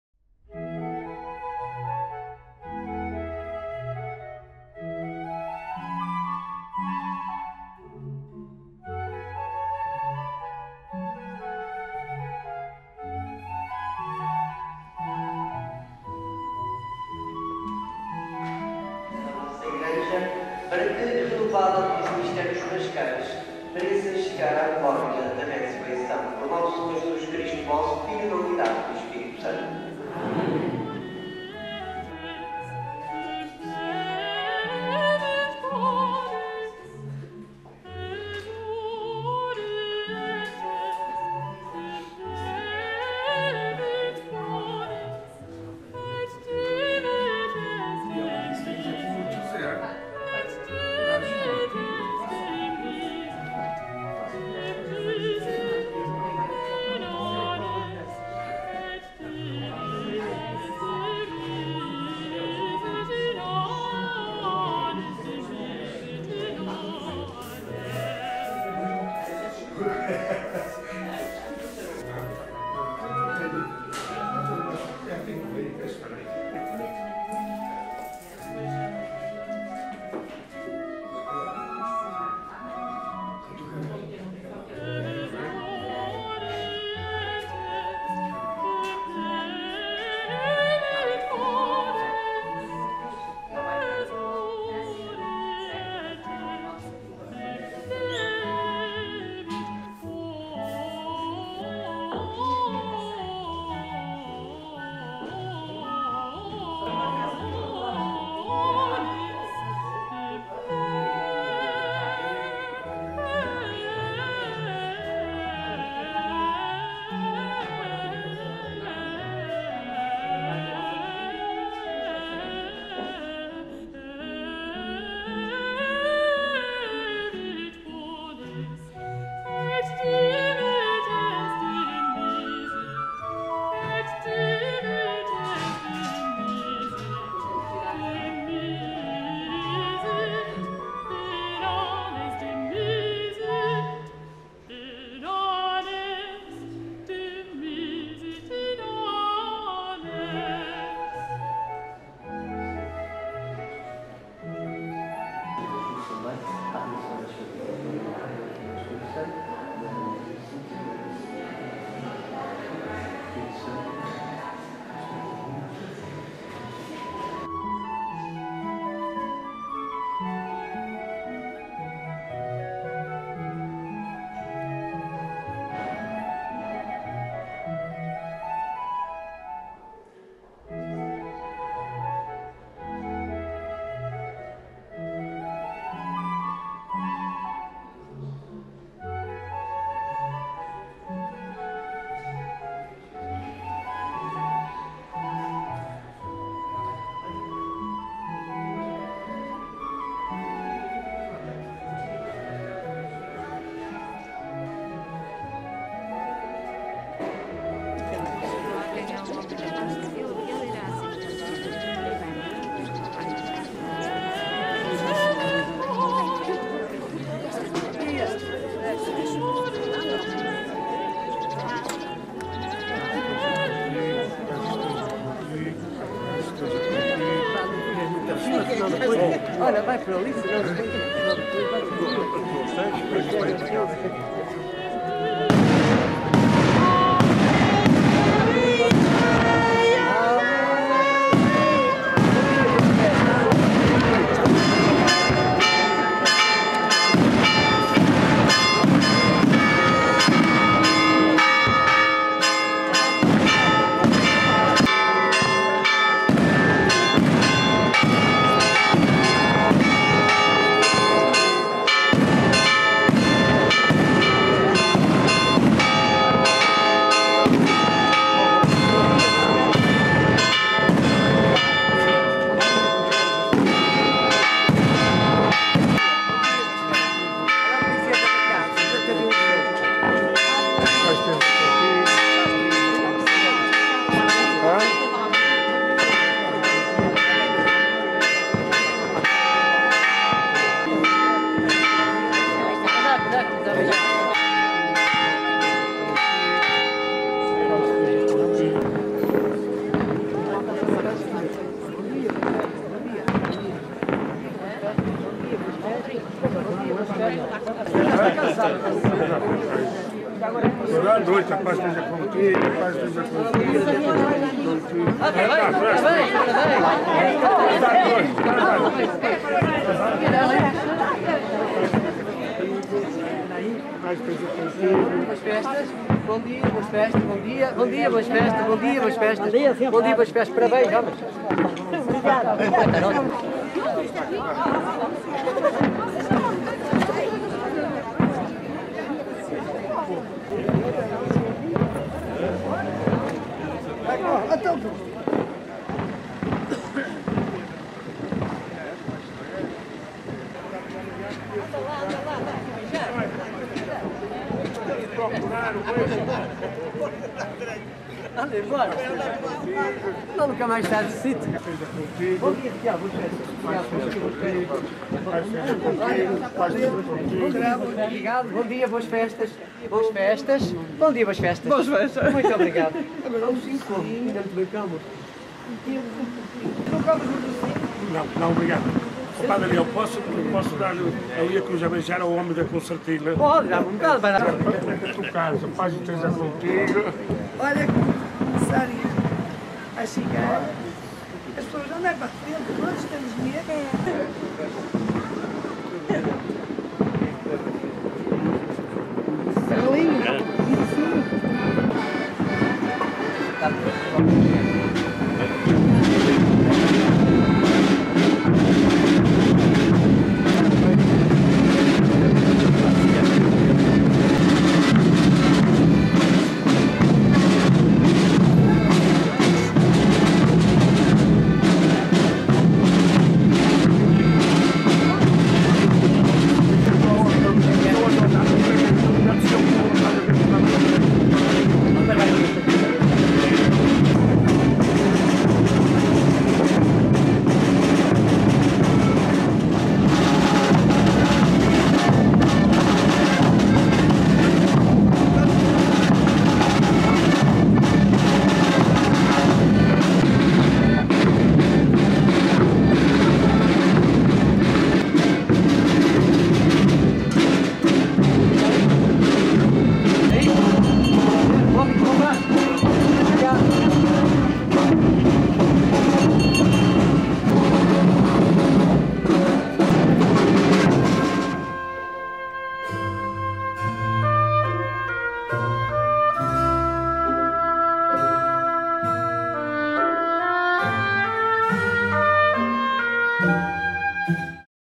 Domingo de Páscoa. Saída da Cruz
Domingo, dia 31 de março do ano 2024, dia de Páscoa. Na paróquia de Lanheses o Compasso Pascal sai da igreja. Um momento muito aguardado e participado. Ia começar a Visita Pascal, levar a boa nova às famílias de Lanheses. Convívio, amizade, solidariedade. É dia de Páscoa. Viva a Páscoa.
As tradições nascem e morrem. Que não morra esta.